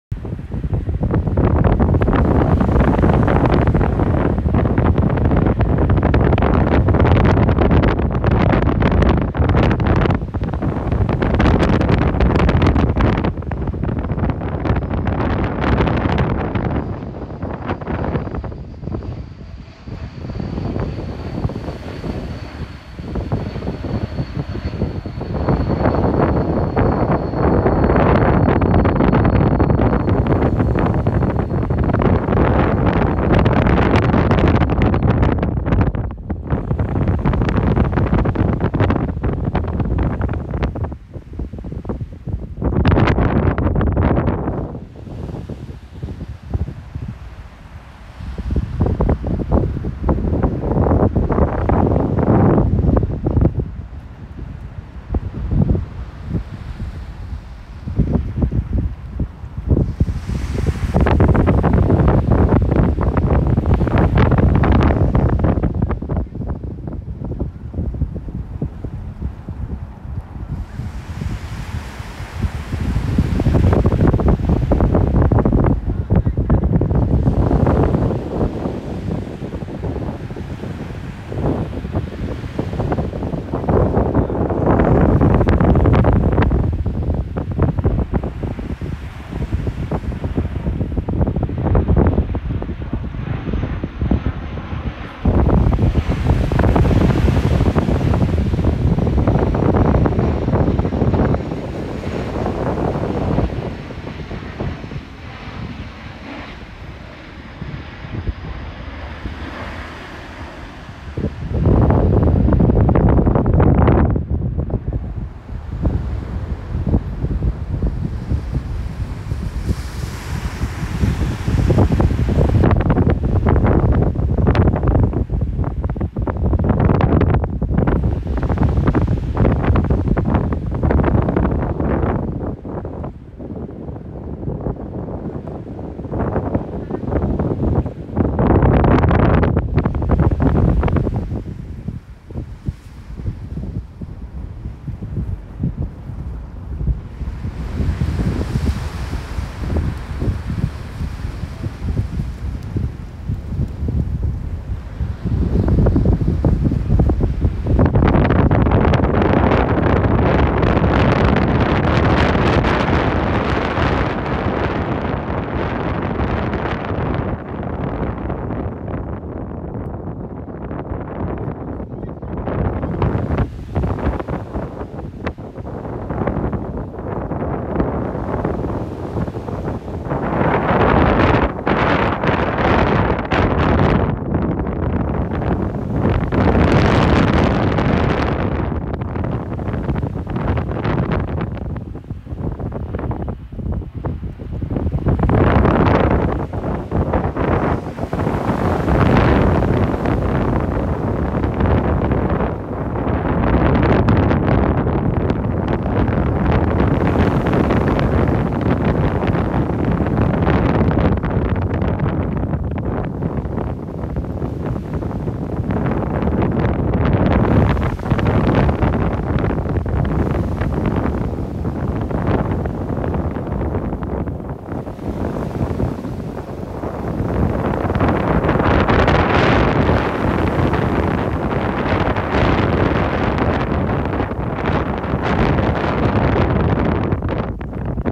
Apple iPhone 13 Pro Max ProRes 422 HQ Filmic Pro app Ultra wide camera lens Revere Beach scenery
Did some test clips at Revere Beach using the telephoto, regular standard wide and ultra wide cameras. This clip has the camera with the ultra wide lens. You can see the beach, sand, buildings, condos, blue sky, clouds, seagulls / birds and planes.

It was in the afternoon and very windy.